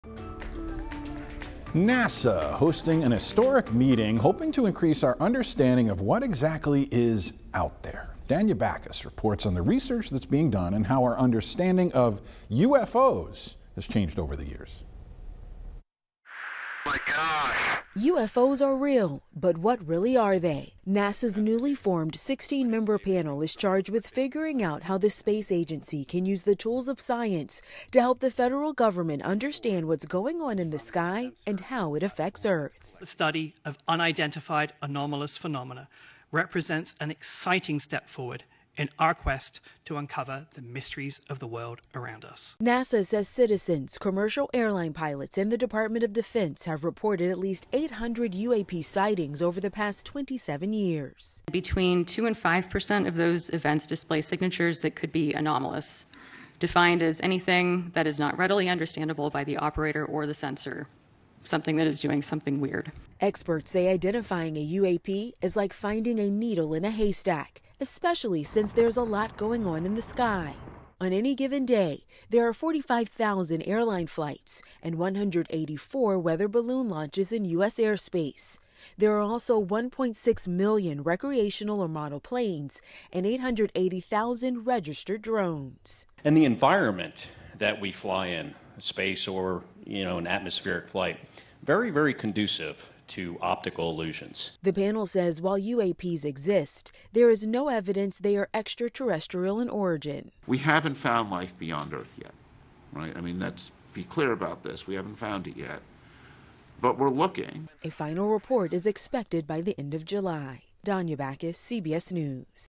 NASA holds its first public meeting on UFOs
A group of 16 scientists and other experts is digging into unclassified information to "separate fact from fiction."